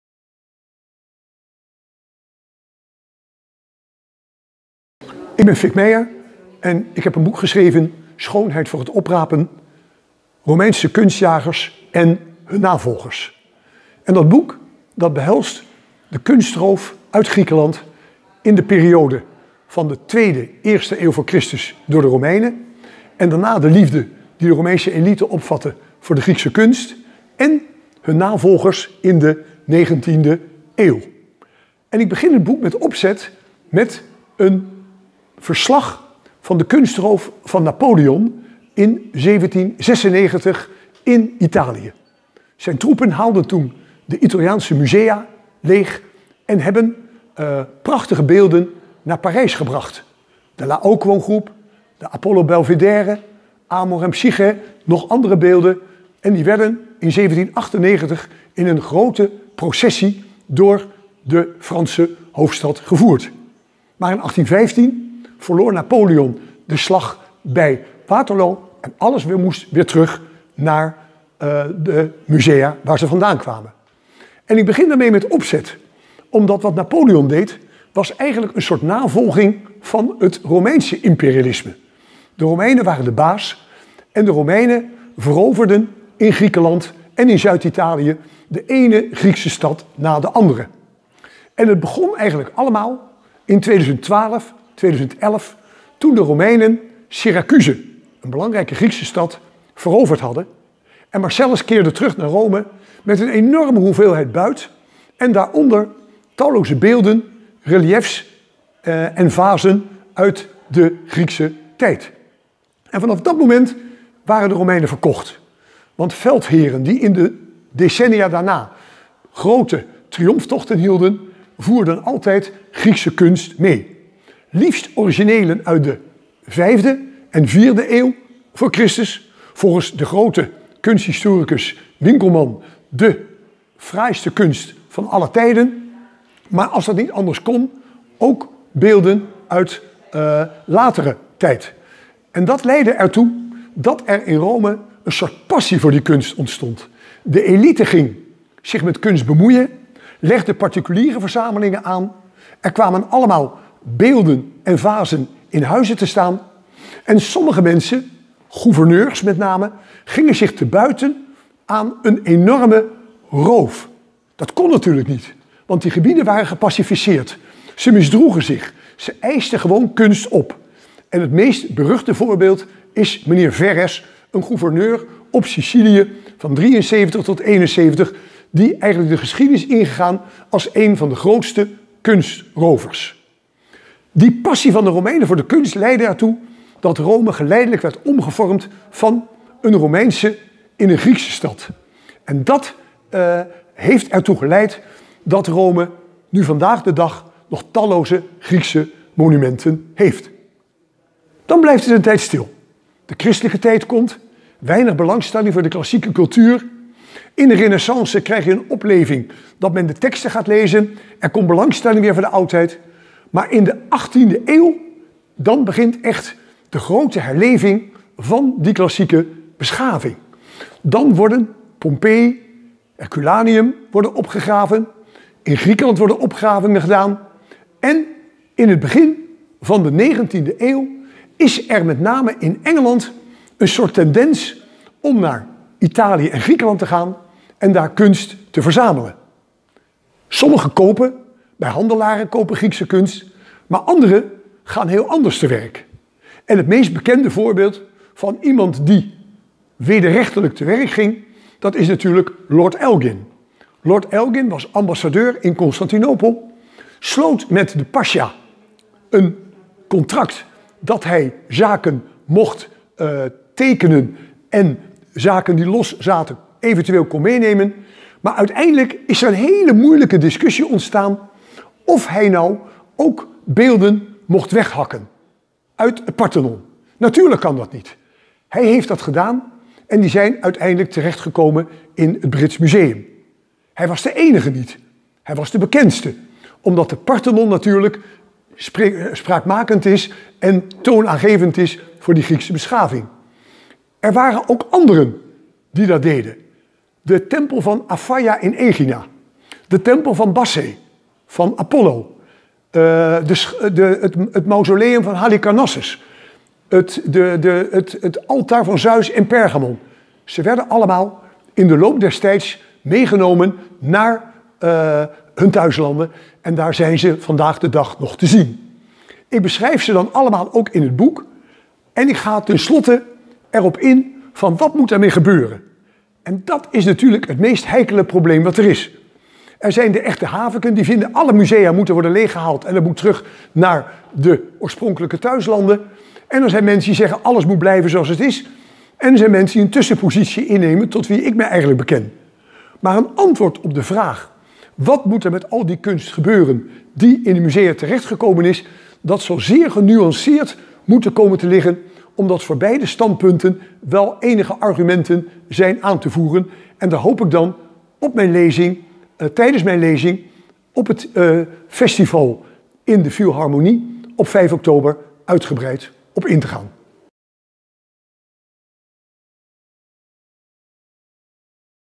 Fik Meijer over kunstroof
Tijdens het Geschiedenis Festival op 5 oktober vertelt Fik Meijer alles over kunstroof van de klassieken. Info en tickets